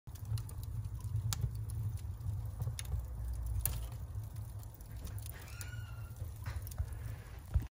Wood fired oven
Watching my wood fired oven burn